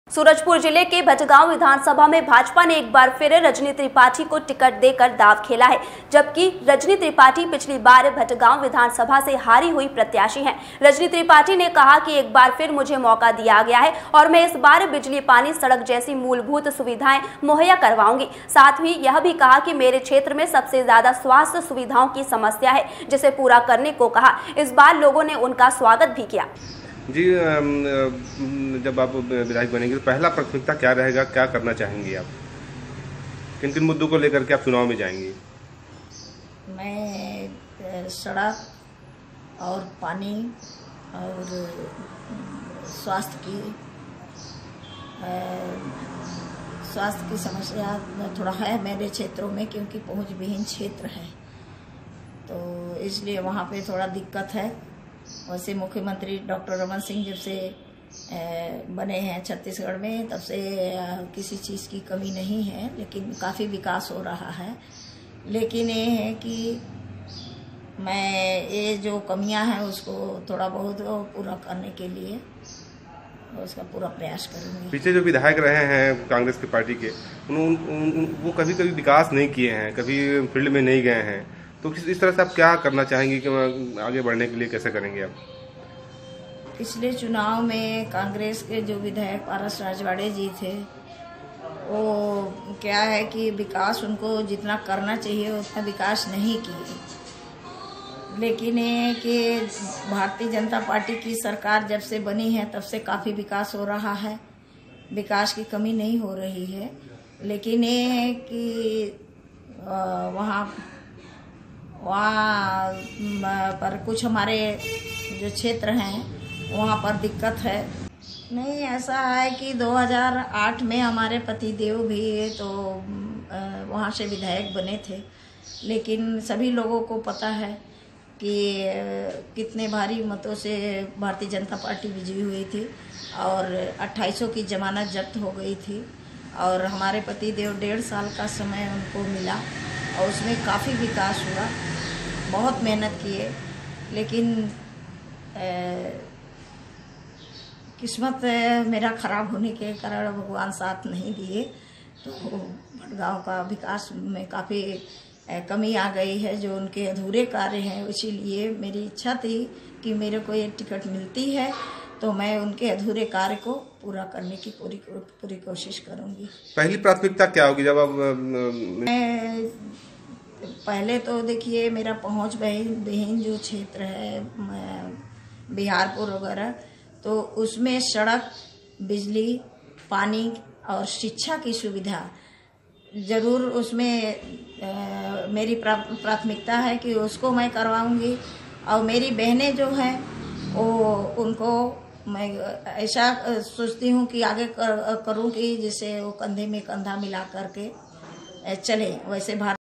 सूरजपुर - भटगांव विधनसभा से रजनी त्रिपाठी होगीं उम्मीदवार - FAST INDIA 24
सूरजपुर जिले के भटगांव विधनसभा मे भाजपा ने एक बार फिर  रजनी त्रिपाठी को टिकट दे कर दाव खेला है, जबकी रजनी त्रिपाठी पिछ्ली बार  भटगांव विधनसभा से हारी हुई प्रत्याशी हैं। रजनी त्रिपाठी ने कहा कि एक बार फिर मुझे मौका दिया गया है और मै इस बार बिजली पानी सड़क जैसी मुलमूलभूत सुविधायें मुहैया कराऊगी साथ ही यह भी कहा की मेरे क्षेत्र में सबसे ज्यादा स्वास्थ्य सुविधाएं की समस्या है जिसे पुरा करने को कहा। इस प्रकार लोगो ने उनका स्वागत किया।